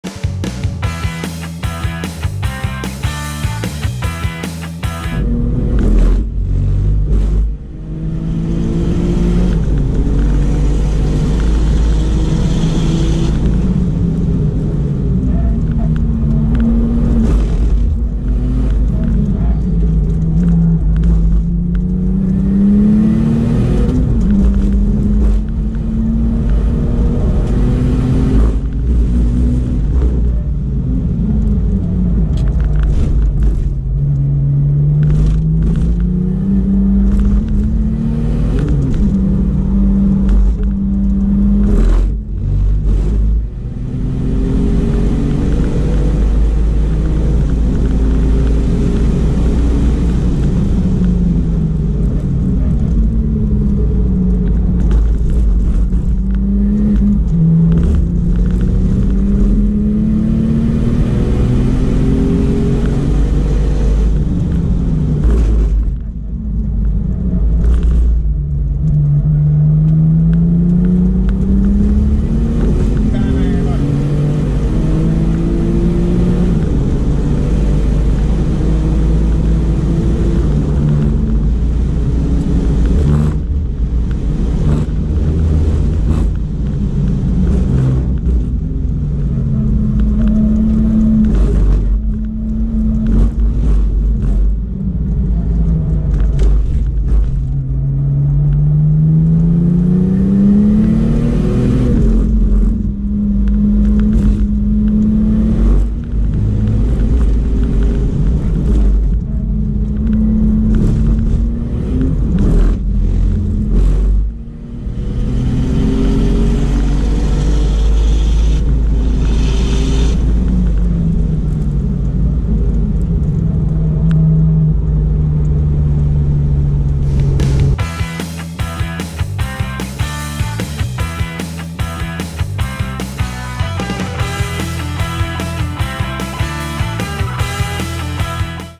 Porsche 996 GT2 Misano Best 1:46,9
Seconda tappa del categoria Street GT Classic, pneumatici Pirelli Trofeo R.